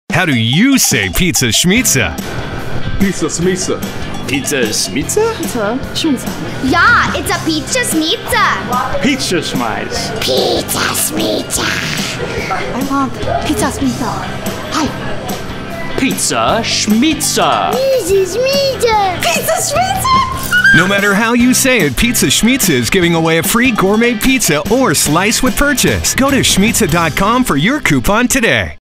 Pizza Schmizza Commercial
A bizarre fever dream commercial from a small pacific northwest pizza chain. Truly the pizza Portlanders deserves.